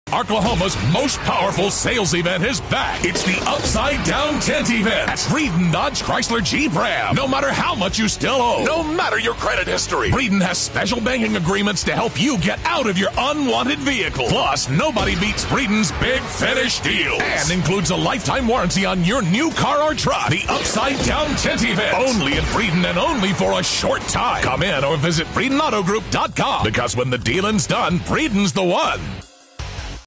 Upside Down Tent Event is BACK at Breeden Dodge Chrysler Jeep RAM
That's right the Upside Down Tent Event is back at Breeden Dodge Chrysler Jeep RAM and its better than ever.

BDCJ 3656